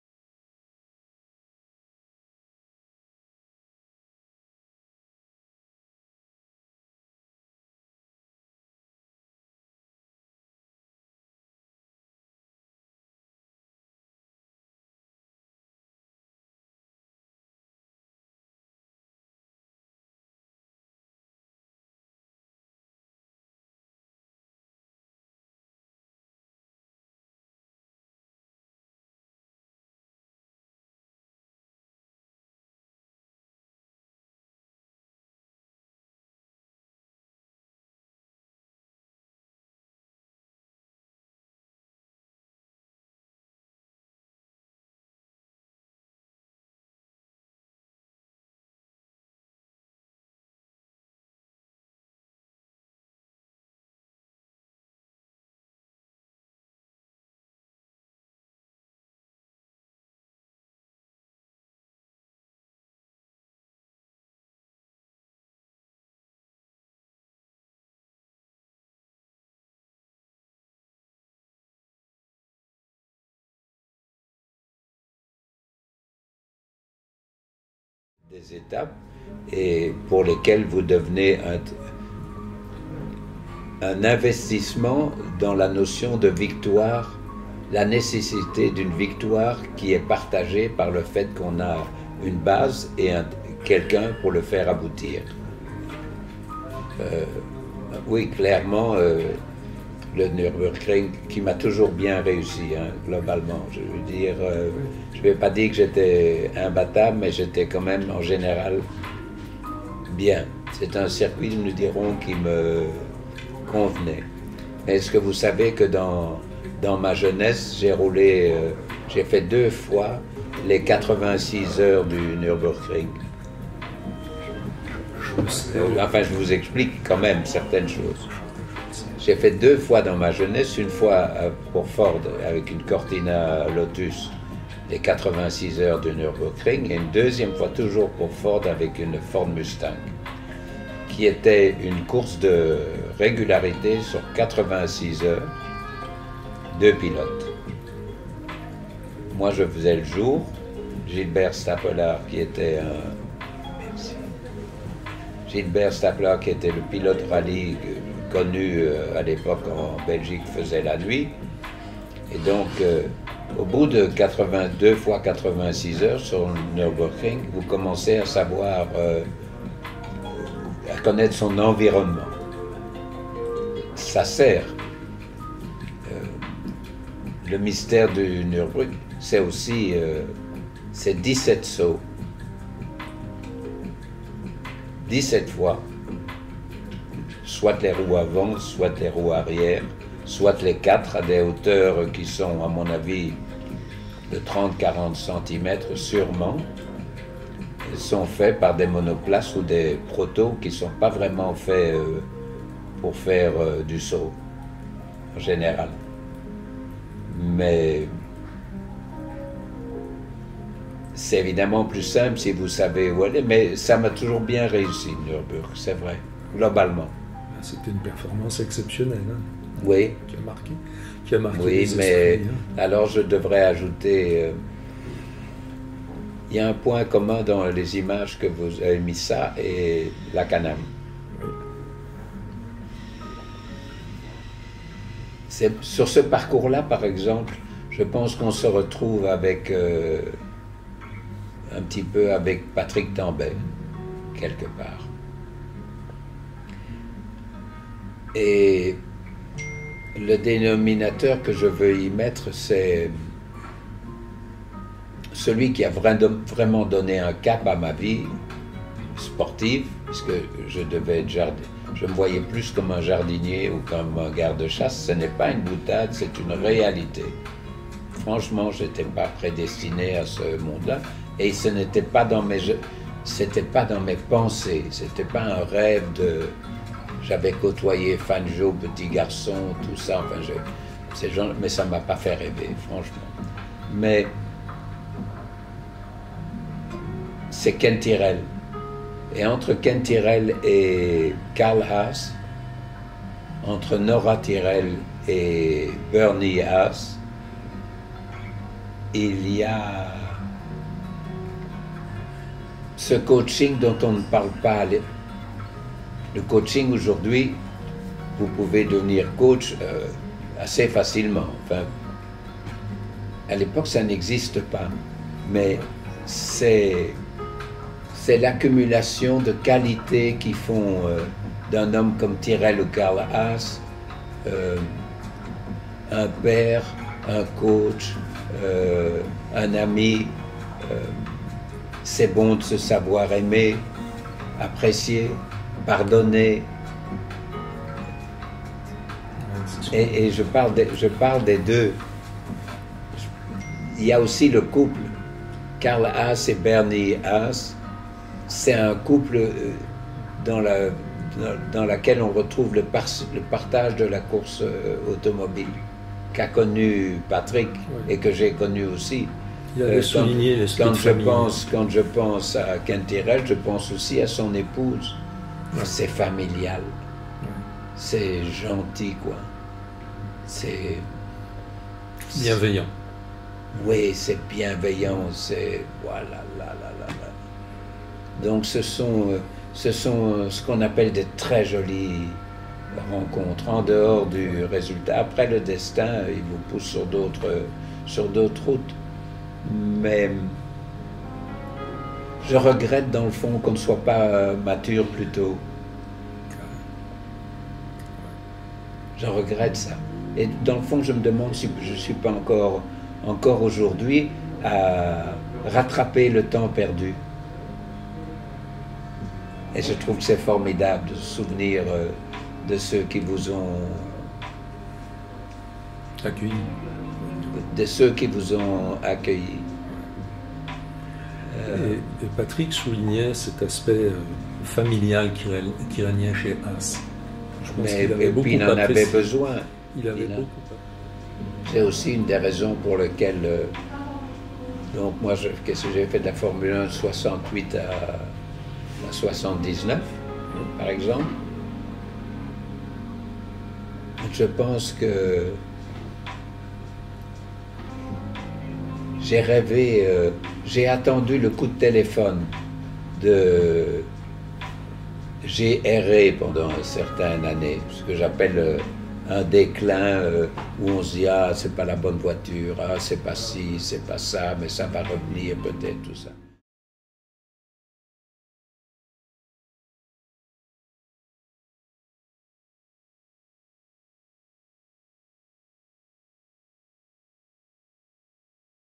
Classic COURSES, Jacky Ickx à coeur ouvert - Le Nürburgring - Tyrrell - Haas (4)
Lors de l'entretien que Jacky Ickx a accordé à Classic-Courses, nous avons découvert un philosophe derrière le formidable pilote. Les premières vidéos publiées en attestent. Il a donc été difficile de l'amener sur ses exploits en piste. Il a eu, pour nos lecteurs, la délicatesse d'en partager certains. Mais toujours avec ce recul humaniste qui rend ses propos si différents et sa personnalité si attachante.